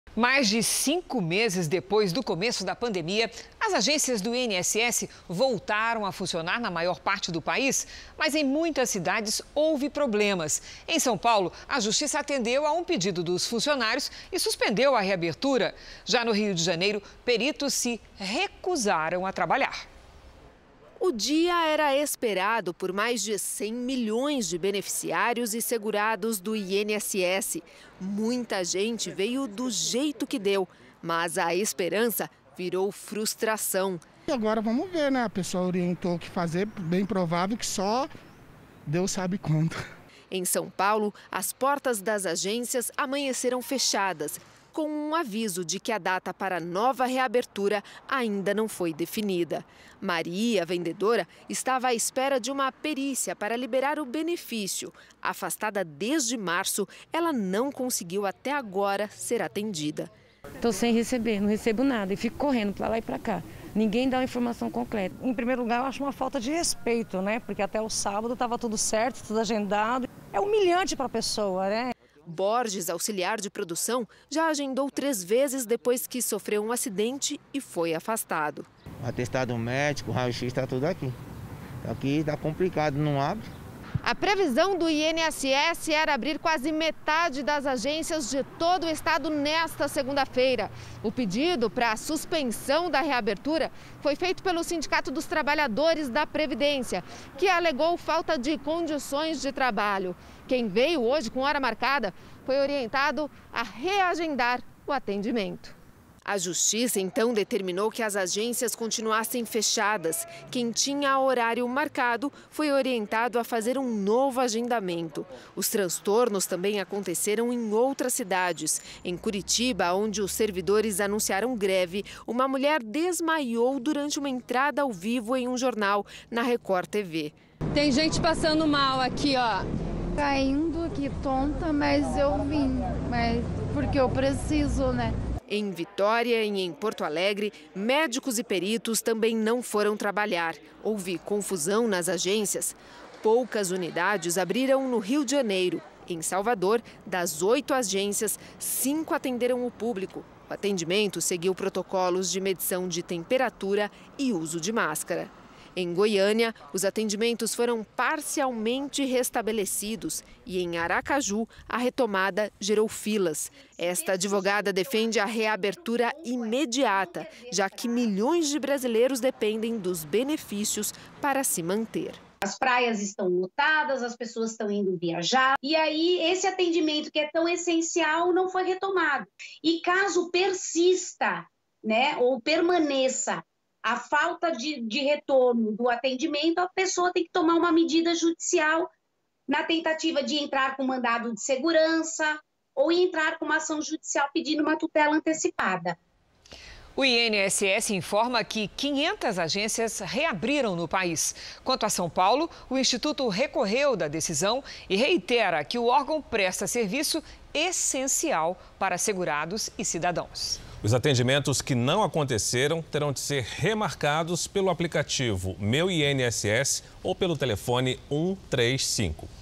Justiça suspende reabertura das agências do INSS em São Paulo
Após mais de 150 dias fechadas em função da pandemia, agências do INSS voltaram a abrir na maior parte do país com atendimento presencial. Em São Paulo, a Justiça acatou um pedido dos funcionários e suspendeu a reabertura. Em Curitiba, servidores anunciaram greve enquanto médicos e peritos se recusaram a trabalhar em Vitória e em Porto Alegre.